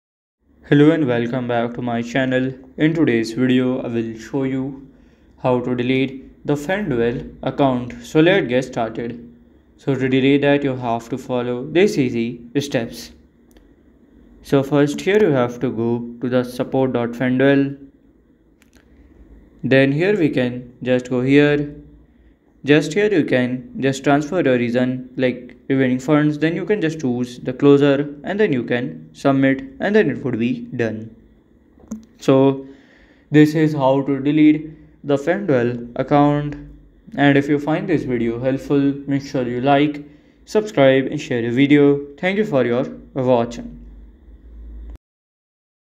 ✅ How To Delete Fanduel Account (Full Guide)
In this guide, learn how to delete your FanDuel account with simple step-by-step instructions. Follow the process to permanently close your account and manage your data.

Key Learning Points:
• Deleting your FanDuel account
• Step-by-step guide to account closure
• Managing your account and personal data on FanDuel